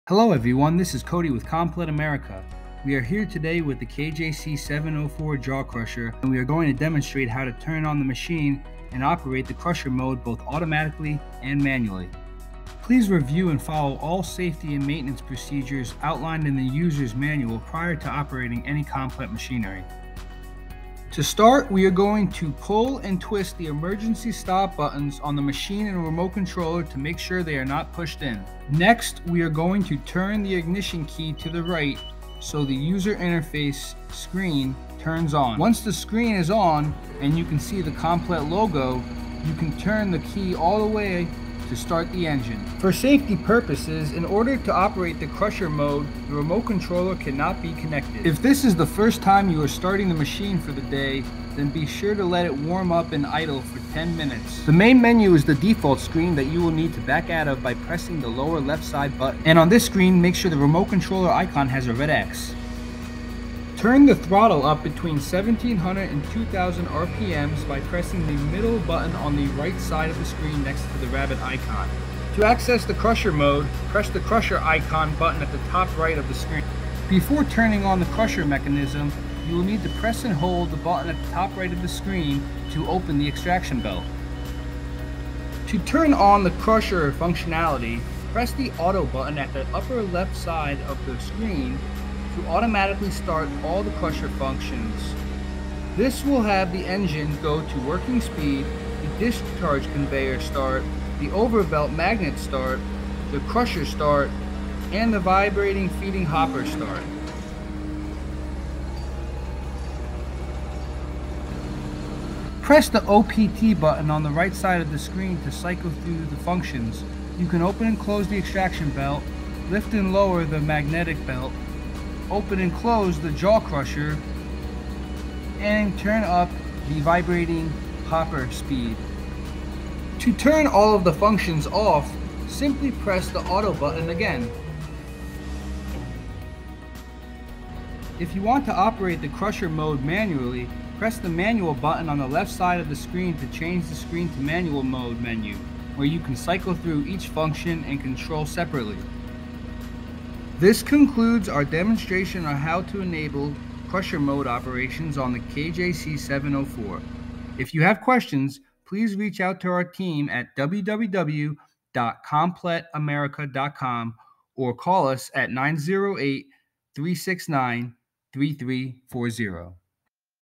Komplet K-JC704 Jaw Crusher - How To Operate Crusher Mode With Display Screen
This video has been created to demonstrate how to turn on the K-JC704 machine and operate the crusher mode both automatically and manually. Please review and follow all safety and maintenance procedures outlined in the user’s manual prior to operating any Komplet machinery.

PLEASE NOTE: The automatic crushing mode can be turned on using the remote controller as well. Please watch this video